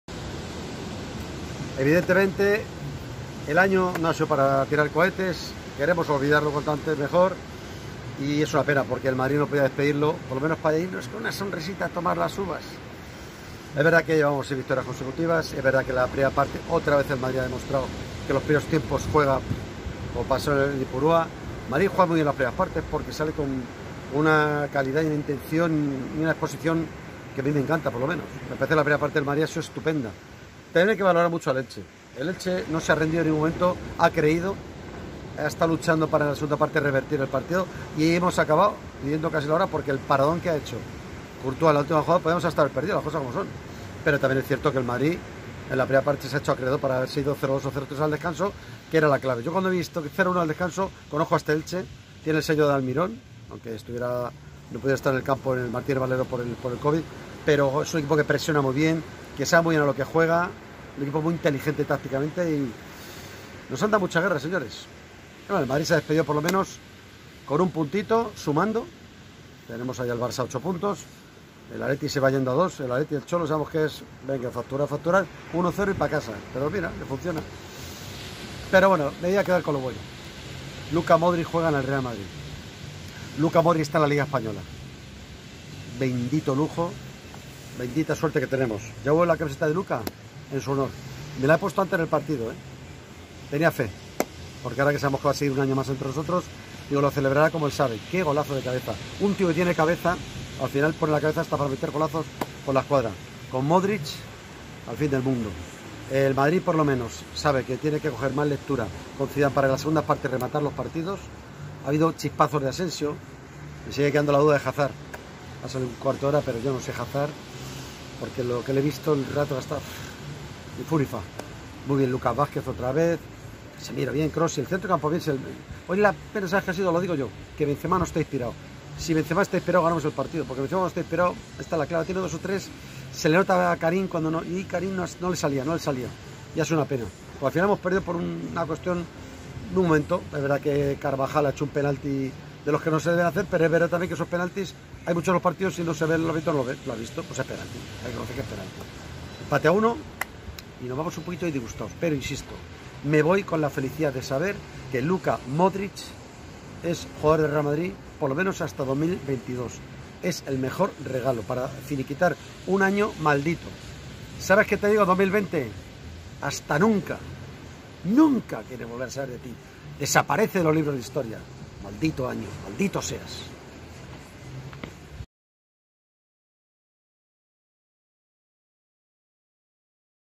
ELCHE 1 REAL MADRID 1 | El análisis de Roncero del último partido del año del Madrid | Diario AS
Roncero analiza el último partido del año del Madrid. A pesar del empate se muestra feliz por el balance de año. La camiseta de Croacio en honor a Modric.